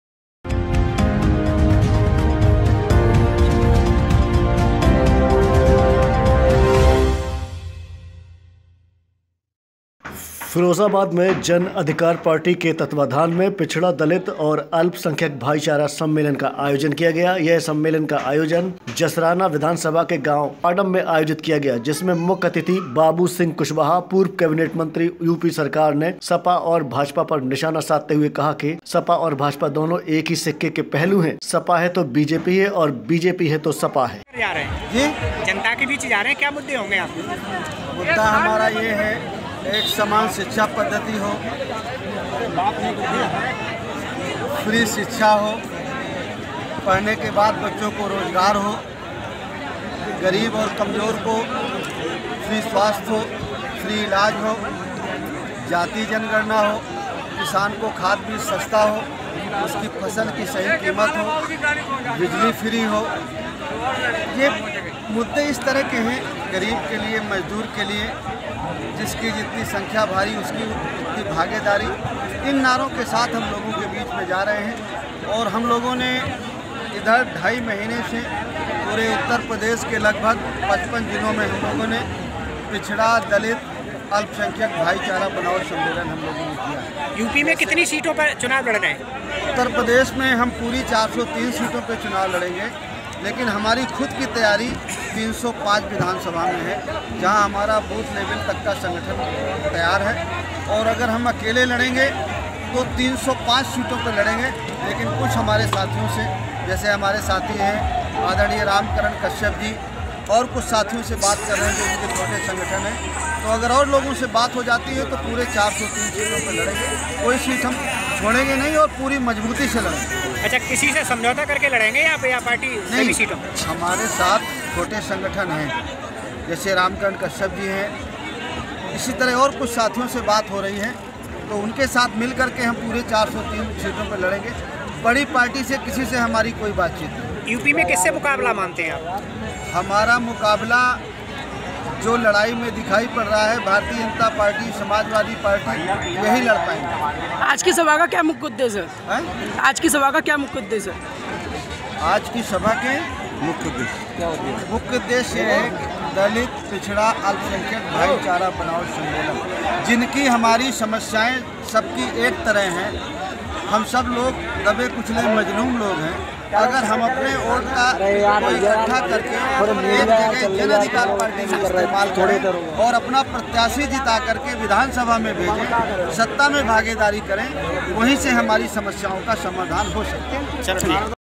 फ़िरोज़ाबाद : जन अधिकार पार्टी के तत्वाधान में पिछड़ा,दलित व अल्पसंख्यक भाईचारा सम्मलेन का आयोजन।
जिसमें मुख्यअतिथि बाबू सिंह कुशवाहा पूर्व केबिनेट मंत्री उत्तर प्रदेश सरकार ने सपा और बीजेपी पर निशाना साधते हुए कहा कि सपा और बीजेपी दोनों एक ही सिक्के के पहलू हैं सपा है तो बीजेपी है और बीजेपी है तो सपा है।